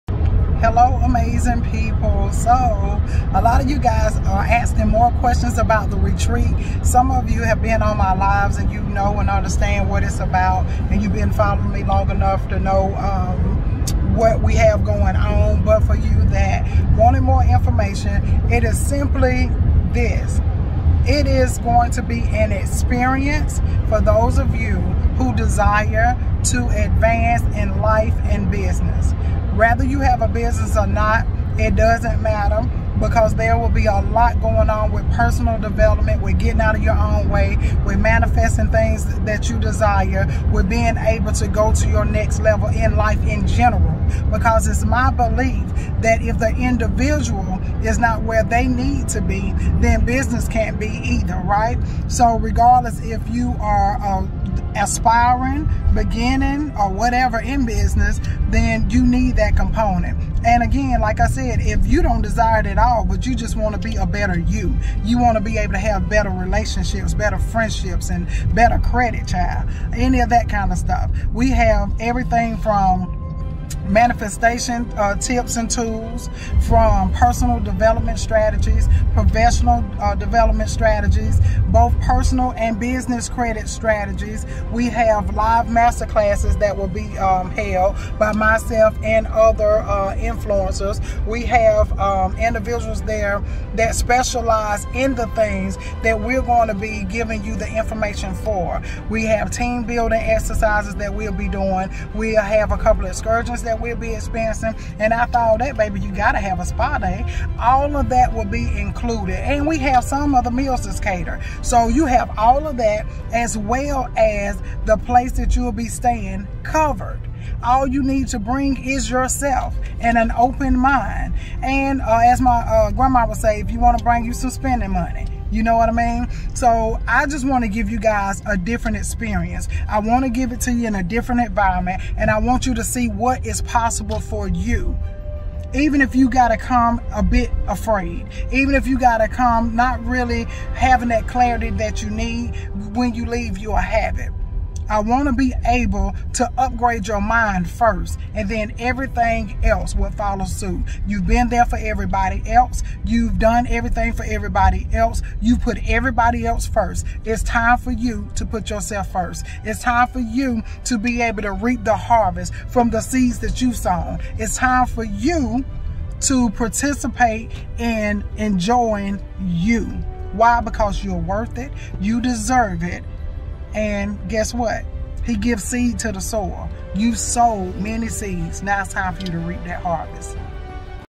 Join myself & Queenpreneur (LaTonya Mims) for the Synergy Retreat.
The Queenpreneur & Nikita B Presents
Synergy Retreat
July 7-10 VIP July 6-10 Orlando Florida
Retreat Experience:
Mindset Mastery
Personal & Professional Growth Strategies
Entrepreneur Success tools
Personal & Business Credit Strategies
Leadership Solutions
Spa Day Experience
Networking & Team excursions
Some Catered Meals
Beautiful lodging & a few more suprises.
Price: $1497 Deposit: $497
Reserve your spot at bit.do/myretreat